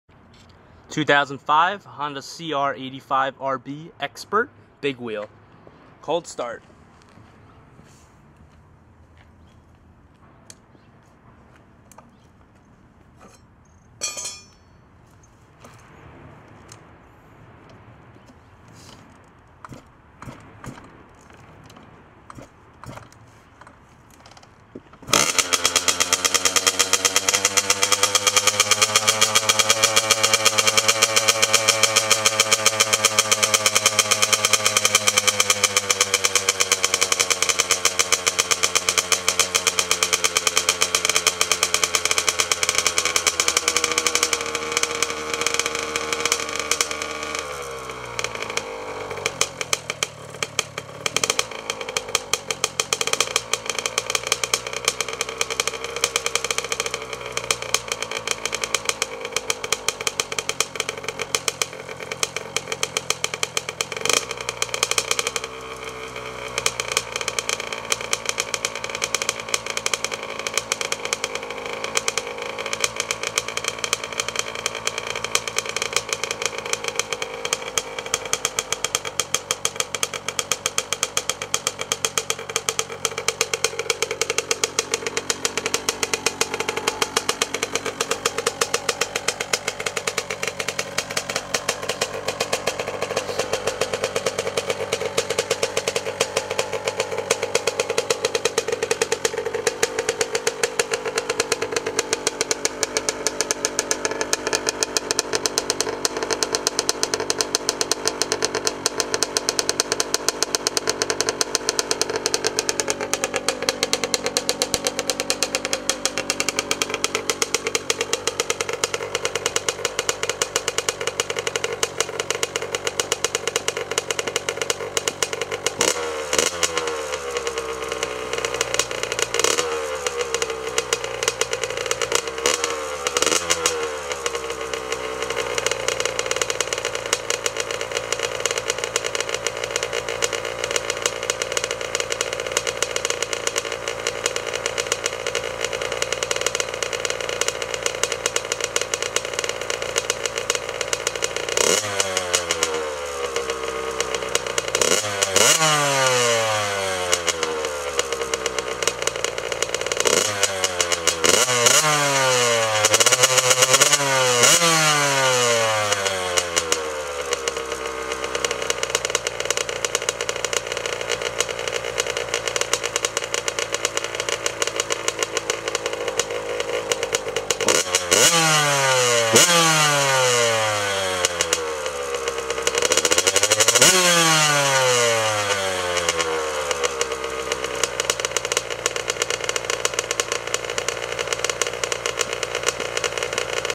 2005 Honda CR85 CR85RB Startup, Exhaust, Walkaround
2005 Honda CR85RB Stock Exhaust, Cold Start and Revs